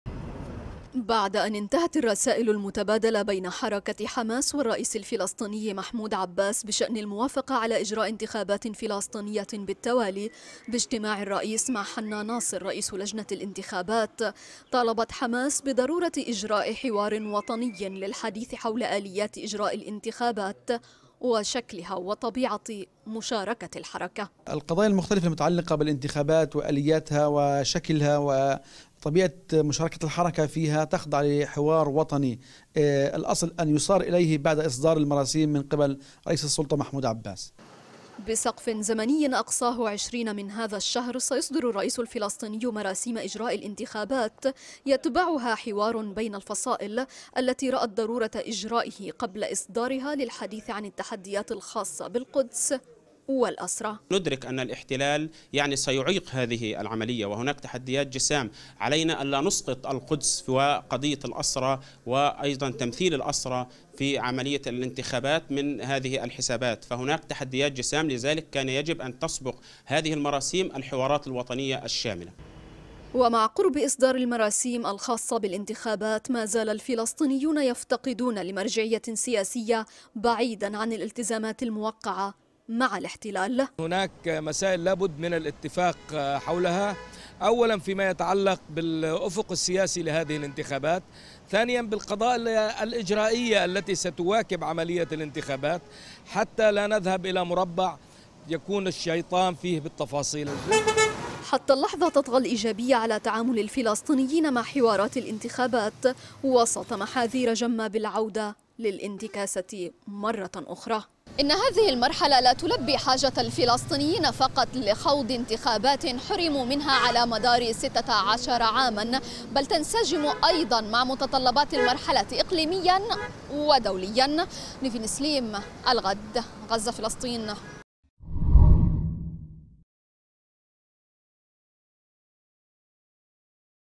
حماس تطالب بضرورة إجراء حوار وطني حول الانتخابات
طالبت حركة حماس والفصائل في غزة بضرورة عقد حوار وطني لبحث التحديات الخاصة بمشاركة القدس والأسرى فى الانتخابات القادمة.

 مطالبة تأتي عقب اجتماع الرئيس الفلسطيني محمود عباس مع رئيس لجنة الانتخابات المركزية حنا ناصر لبحث آليات إصدار المراسيم الرئاسية الخاصة بإجراء الانتخابات.

المزيد من التفاصيل في سياق تقرير مراسلة الغد نيفين اسليم.